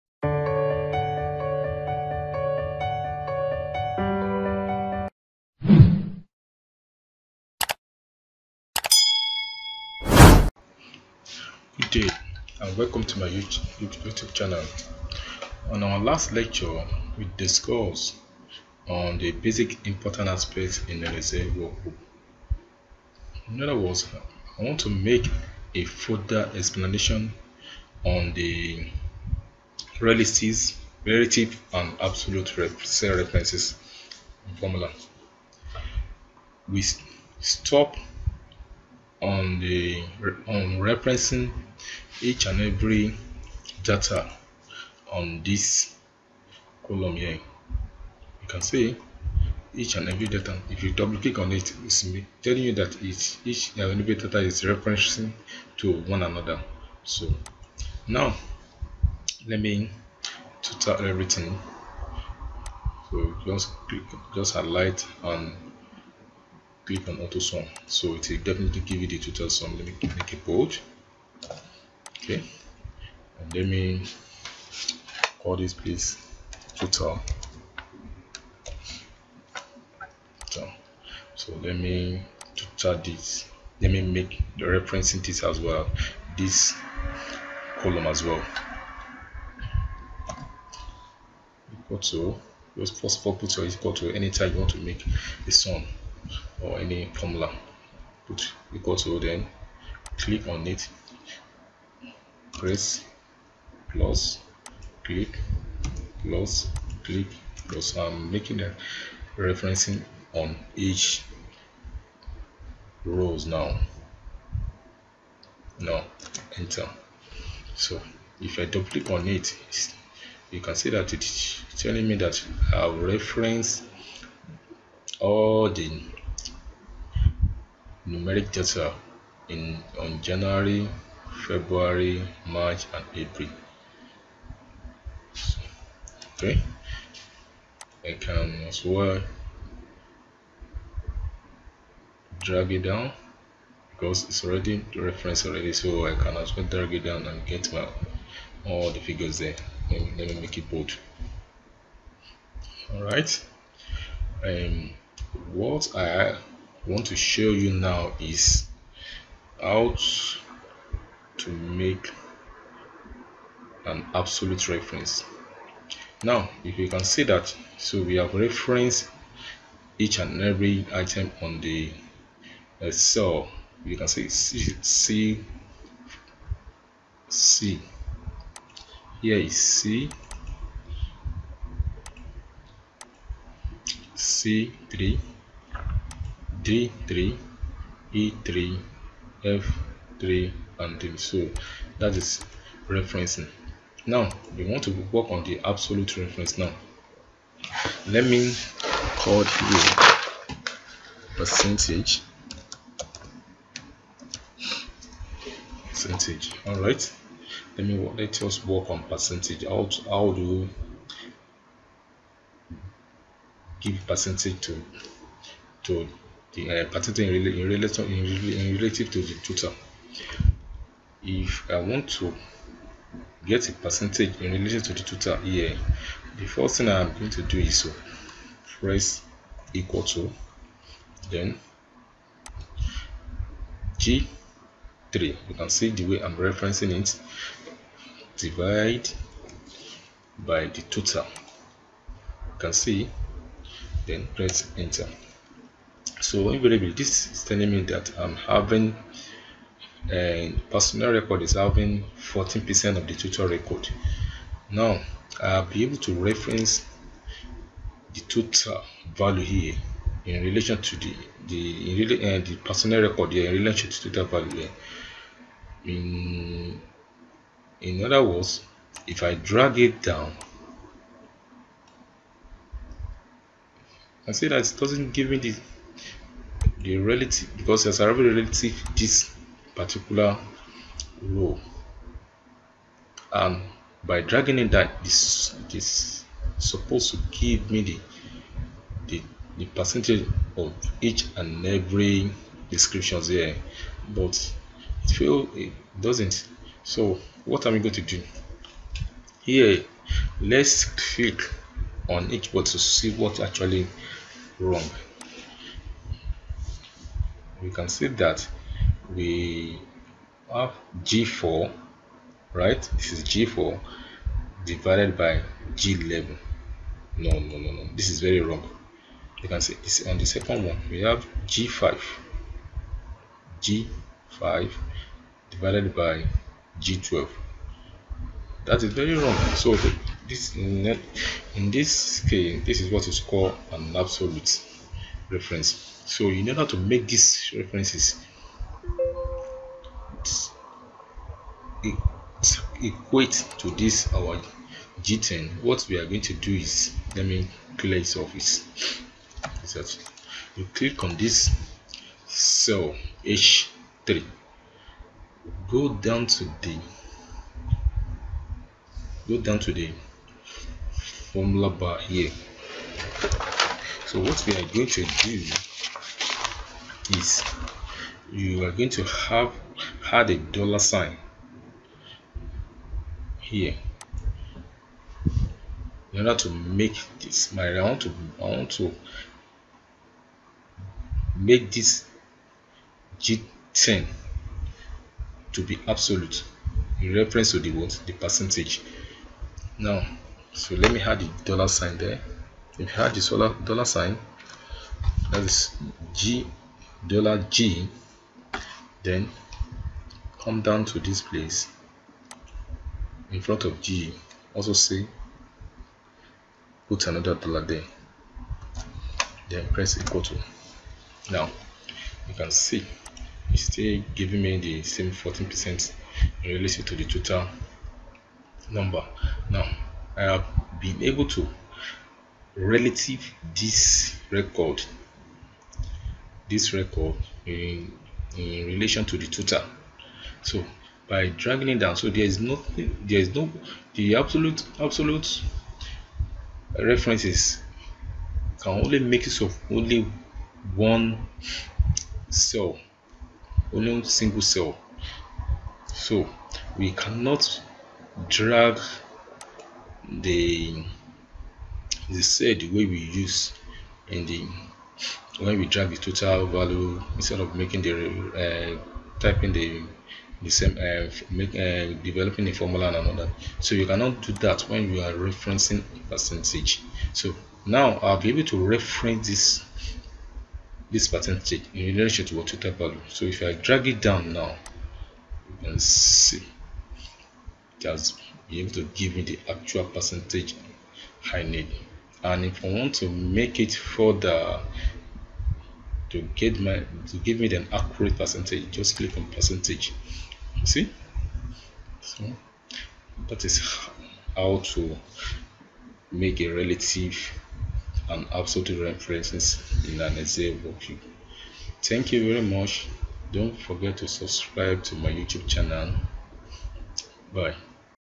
How To Use Relative,Absolute and mixed cell references in Microsoft excel
Learn Quick and easy way on how to use relative absolute and mixed cell references in Microsoft excel Relative Cell References and  Absolute Cell References in an Excel Workbook.

In this video, you will learn:

A cell reference points to a cell on a worksheet and can be used in a formula so that Microsoft Office Excel can find the values that you want. Mixed cell reference occurs when we use both relative and absolute reference